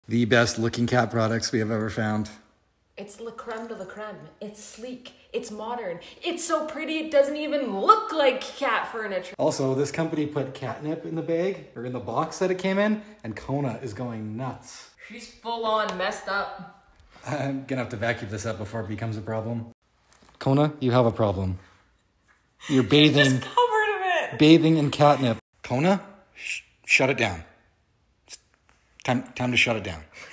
Our bengal cats new cat bed and her new addiction...
Our bengal cat Kona just got new cat furnature and they included cat nip... which makes her loose her cat mind!